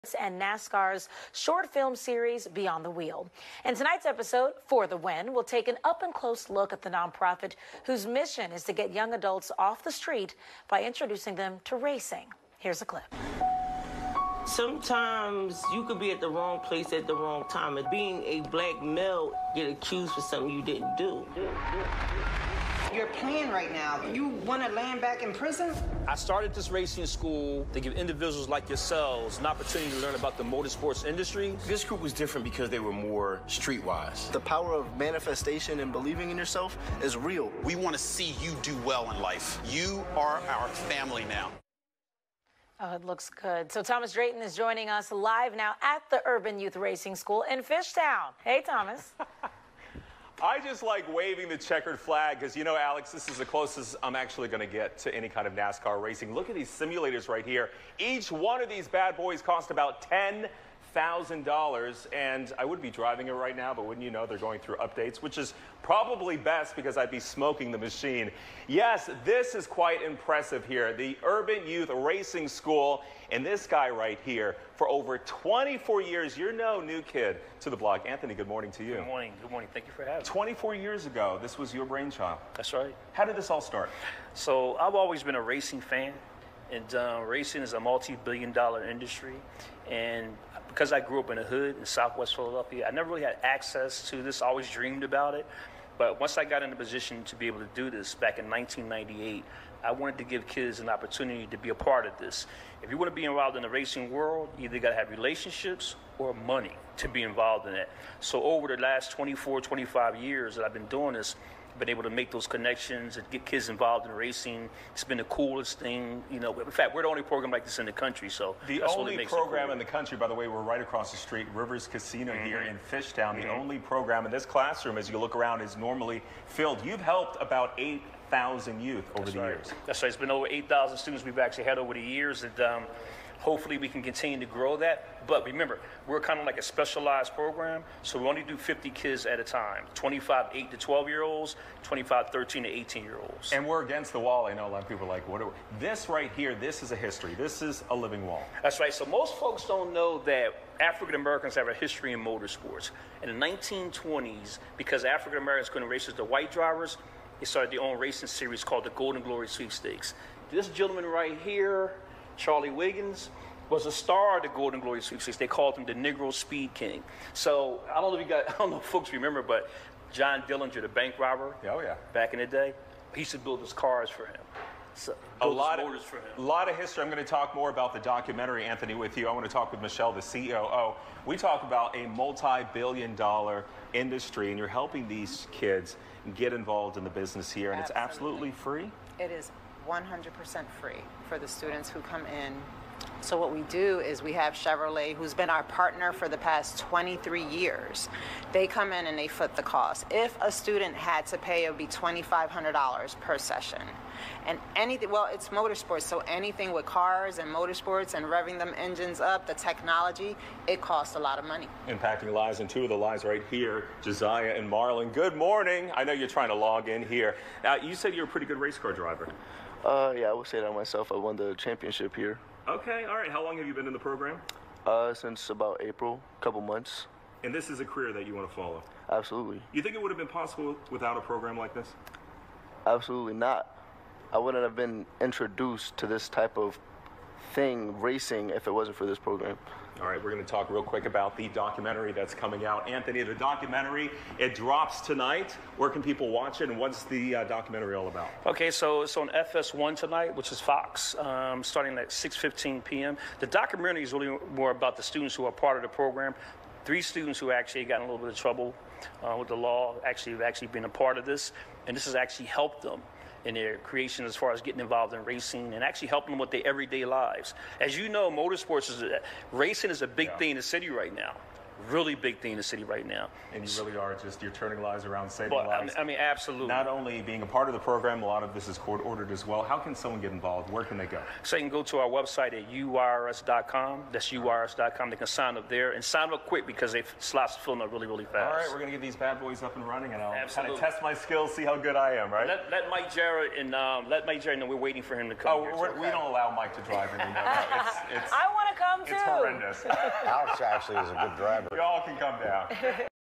Black owned school is training kids to become race car drivers and engineers
You can support The Urban Youth Racing School by following on Instagram @UrbanYouth1